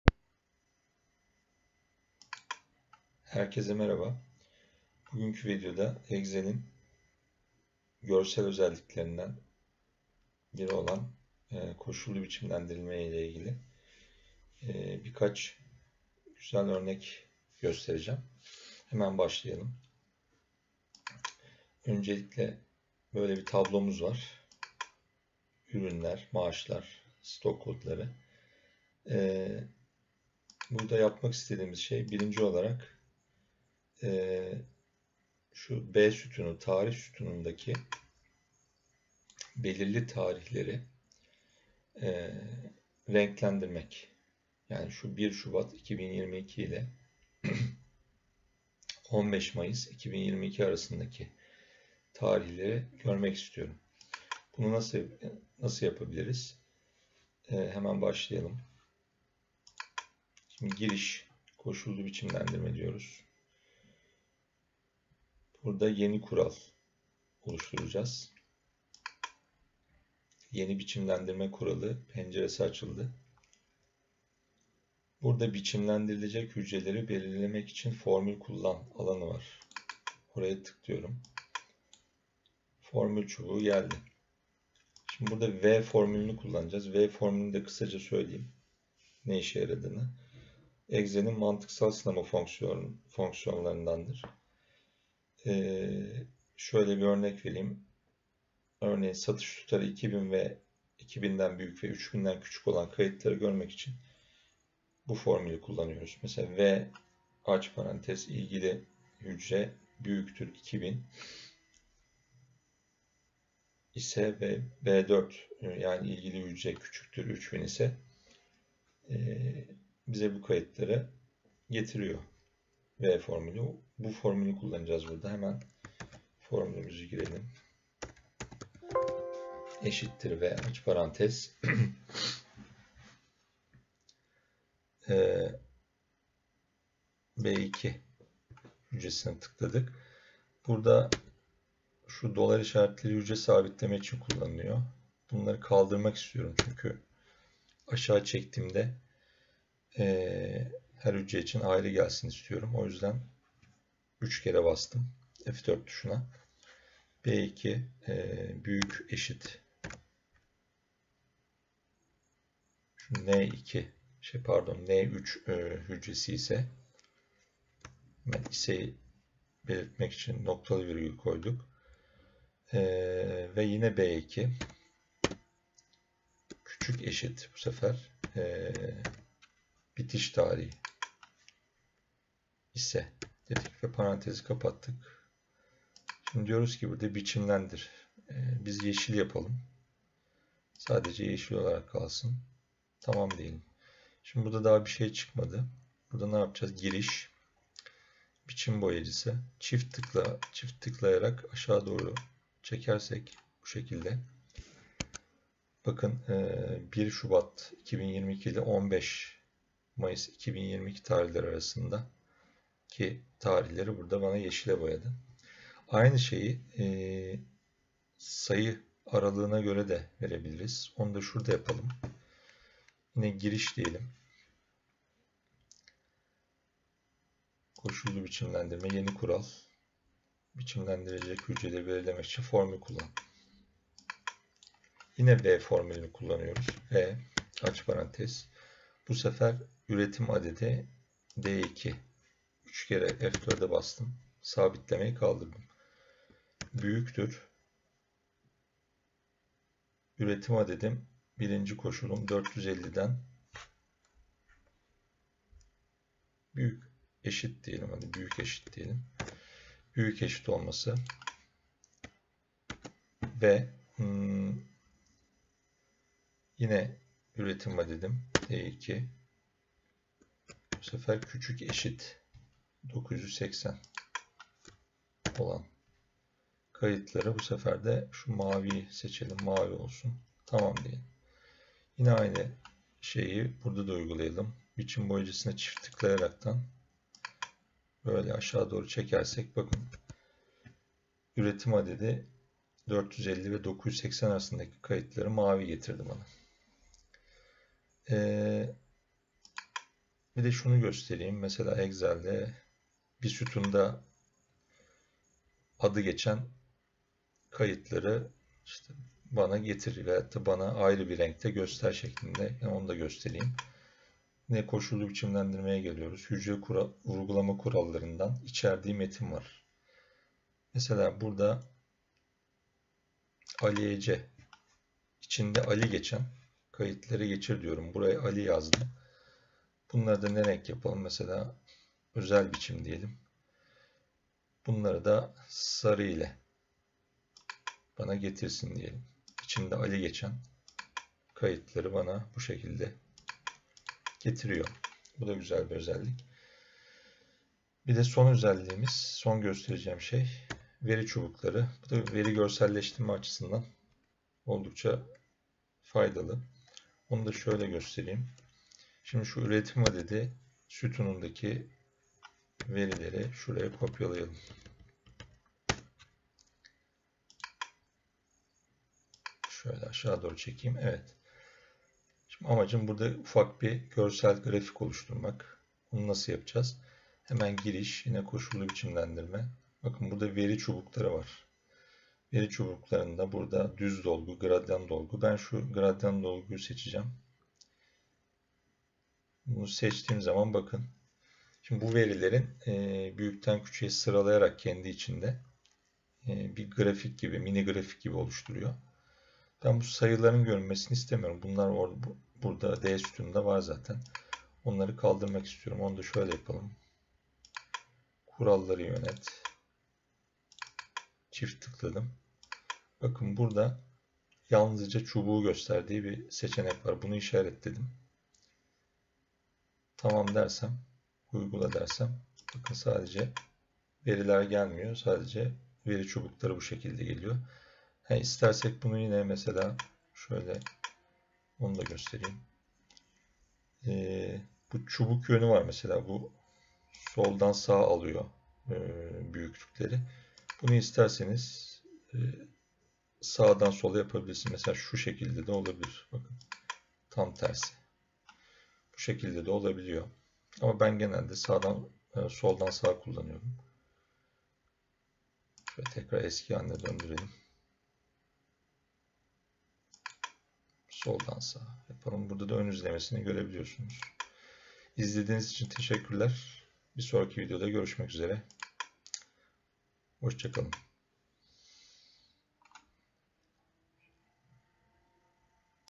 Excel Conditional Formatting Column visualization by date,number ranges
Hello everyone. In this excel training video, I tried to explain the "and" function from the conditional formatting feature in excel, visualization of columns in number ranges, column coloring according to the containing text, data bars and logical test functions.